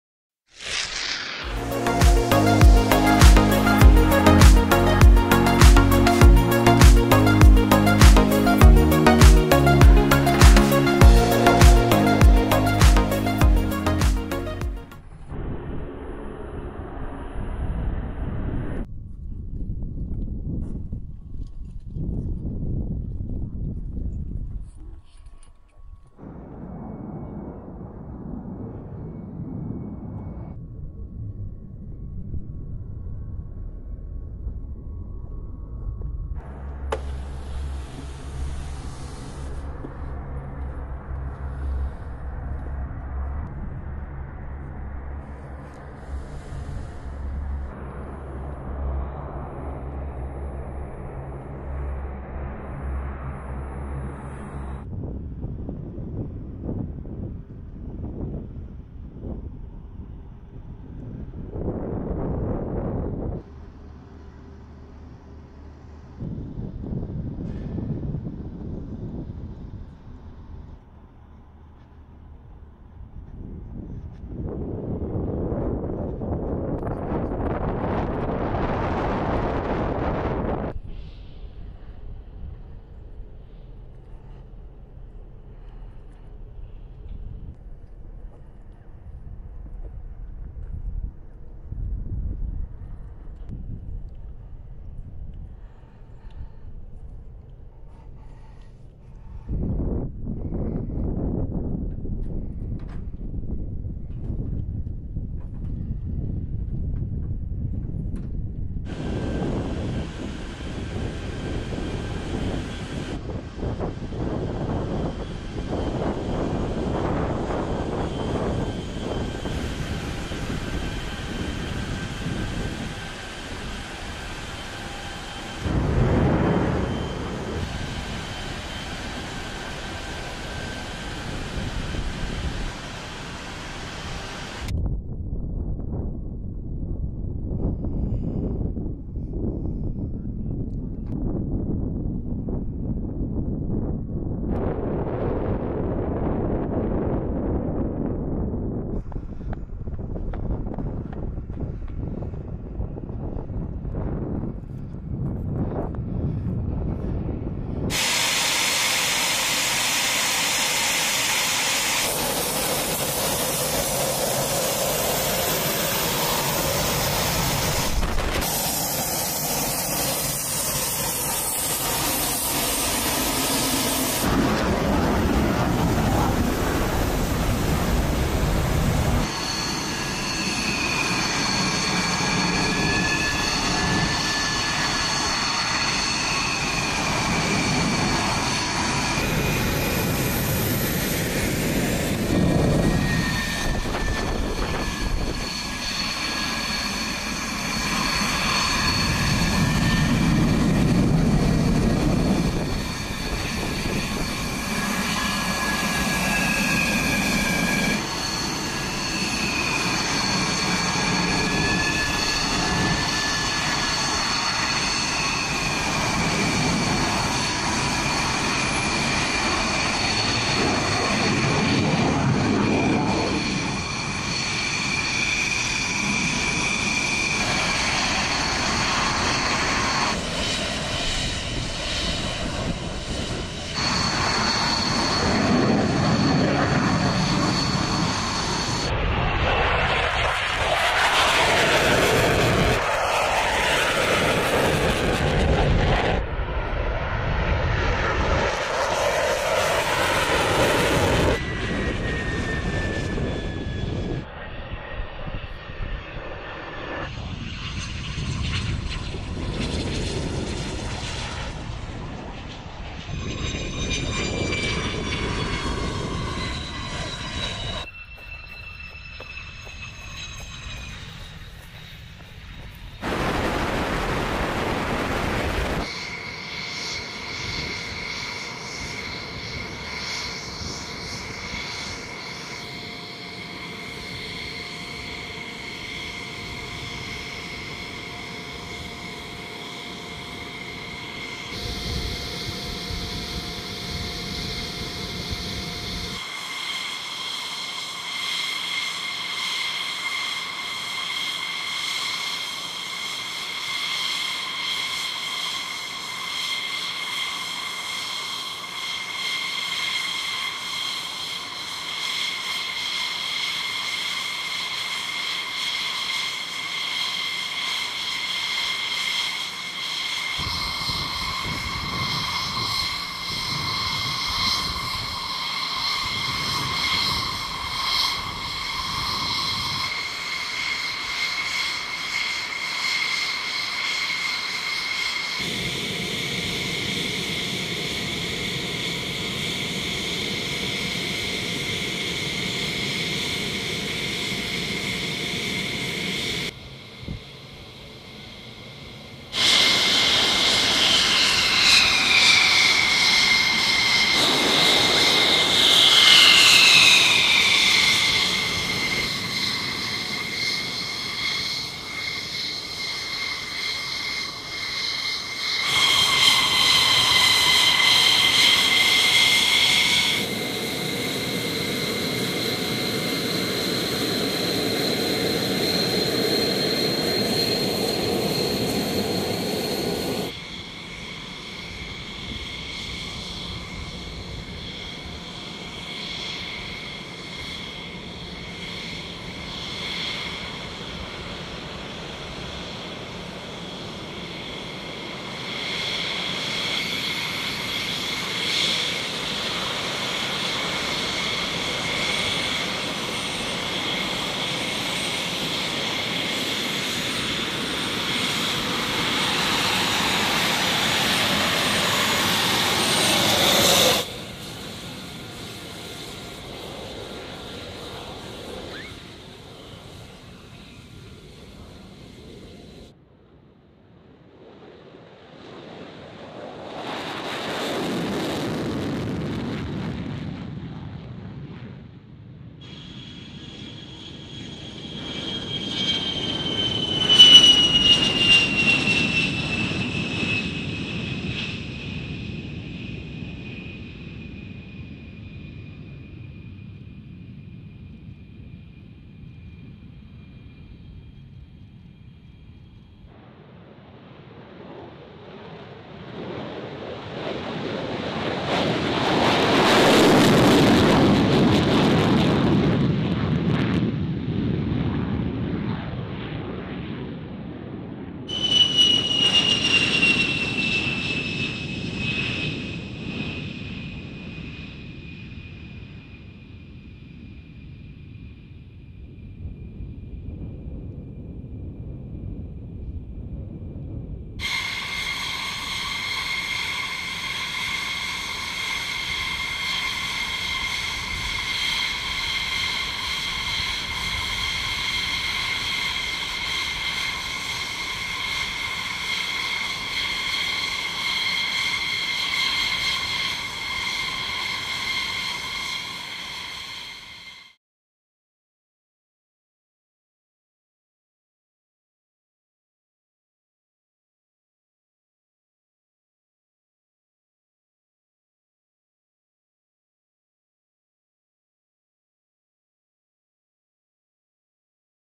Fighter Jets in Special Colour Scheme | NATO Tigers Take the Skies
Welcome to Motorizado for a video about the fighter jets in special colour scheme, NATO Tigers take the skies.

The 2023 edition of the NATO Tiger Meet brought a splash of colour to Gioia del Colle Air Base in southern Italy.

More than 80 aircraft from 13 countries – many of them displaying the vivid, tiger-themed paint jobs that the exercise is famous for – flew a series of tactical training scenarios.

Hosted this year by the Italian Air Force, the NATO Tiger Association was formed in 1961, with the French, UK and US air forces as founding members.

Today, the association boasts 15 NATO Allies and partners as full members.

Footage includes shots of brightly coloured jets taking off and landing.

• The appearance of United States Department of Defense (DoD) visual information does not imply or constitute DoD endorsement

• Credits:
NATO
U.S. Air Force Staff Sgt. Frank Rohrig